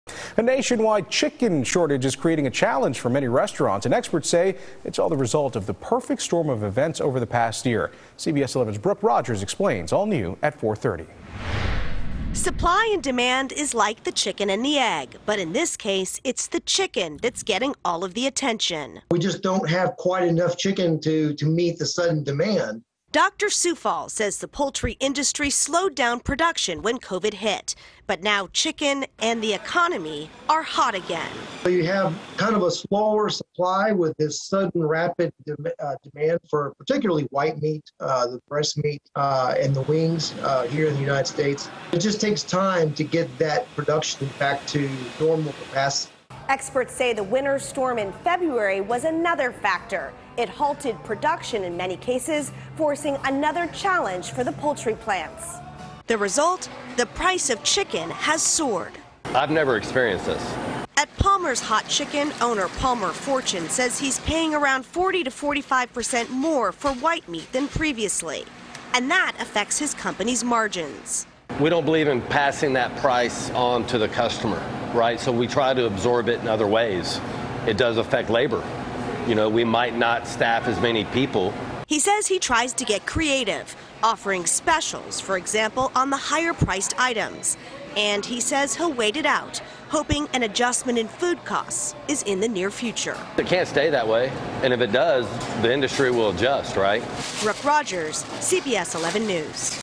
Summertime Shortage: Why You Can't Find Chicken Wings, Breast Meat
A COVID-19 hangover from 2020 is threatening many restaurant owners struggle with shortages and higher prices created by snarled supply chains.